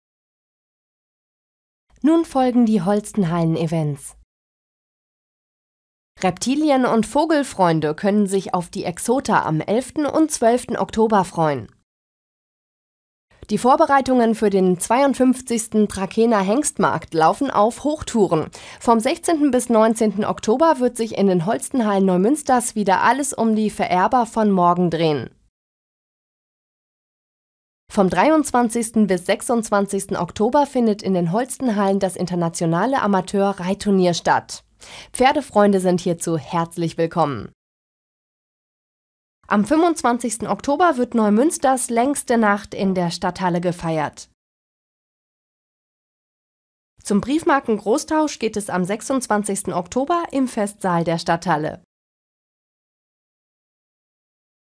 Neumünsters längste Nacht 2014 | Events im Überblick #40 | Holstenhallen Neumünster
Weitere Termine in dieser Ausgabe:
- EXOTA - Vogel- und Reptilienausstellung | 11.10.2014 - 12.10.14
- Internationaler Trakehner Hengstmarkt | 16.10.14
- Holstein International - Internationales Amateur-Reitturnier | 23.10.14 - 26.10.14
-Briefmarkengroßtauschtag 26.10.14